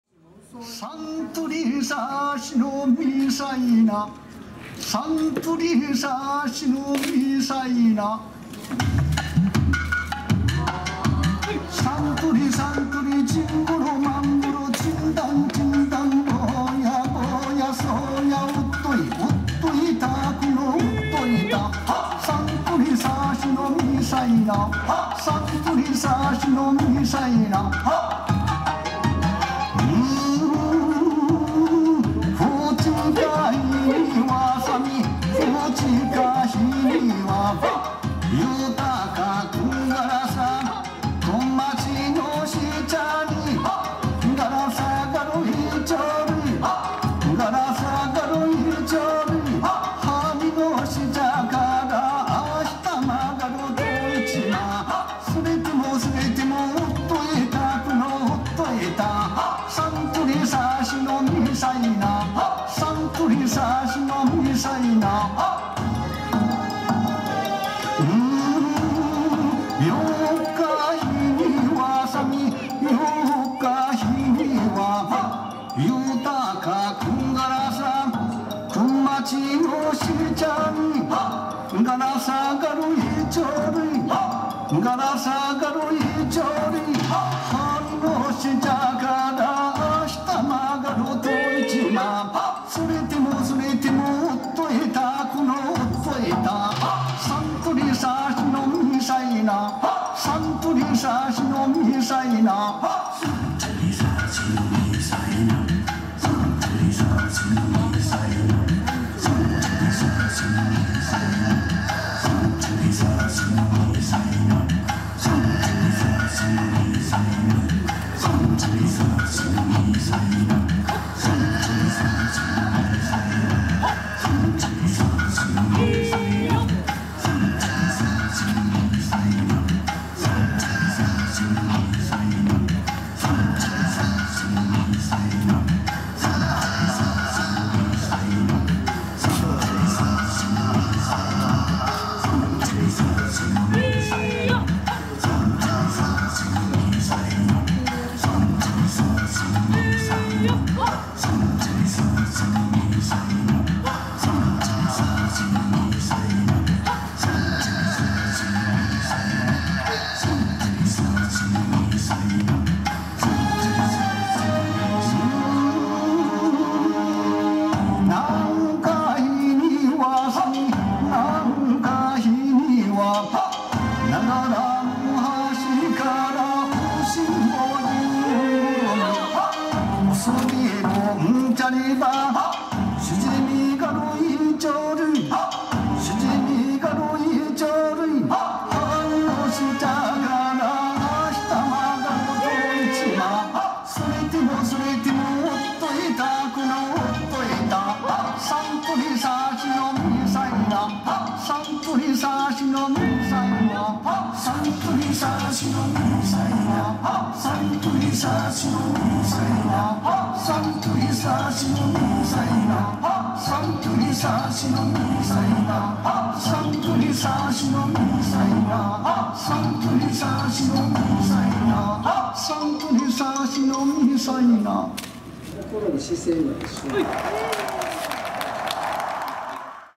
創作太鼓団 すたんぷ (那覇国際通り てんぶす館前広場）
那覇市国際通り 那覇てんぶす館前にて 創作太鼓団 すたんぷによるエイサー演舞が行われました。（November 6 2016)